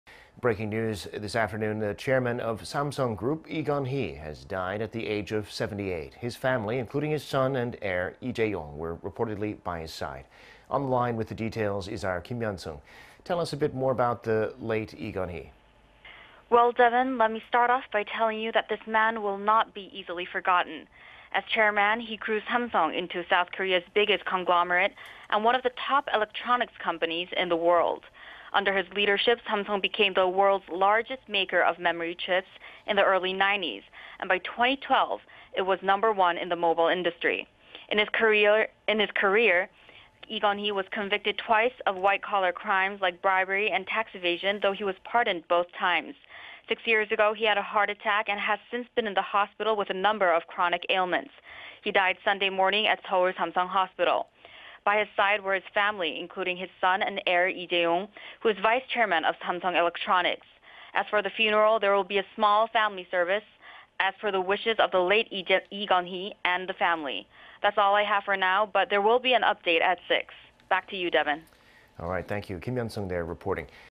Samsung chairman Lee Kun-hee dies at 78
이건희 78세에 별세

Breaking news this afternoon  the chairman of Samsung Group, Lee Kun-hee, has died at the age of 78.
His family, including his son and heir Lee Jae-yong were reportedly by his side.
On the line with the details is our Kim Yeon-seung.
Tell us a bit more about the late Lee Kun-hee.
Well, Devin, let me start off by telling you that this man will not be easily forgotten.
As chairman, he grew Samsung into South Korea's biggest conglomerate and one of the top electronics companies in the world.
Under his leadership, Samsung became the world's largest maker of memory chips in the early 90s,... and by 2012 it was number one in the mobile industry.
In his career, Lee Kun-hee was convicted twice of white collar crimes like bribery and tax evasion, though he was pardoned both times.
Six years ago he had a heart attack and has since been in the hospital with a number of chronic ailments.
He died Sunday morning at Seoul Samsung Hospital.
By his side were his family, including his son and heir Lee Jae-yong, who is vice chairman of Samsung Electronics.
As for the funeral, there will be a small family service as per the wishes of the late Lee Kun-hee and the family.
That's all I have for now, but there will be an update at six.
Back to you Devin.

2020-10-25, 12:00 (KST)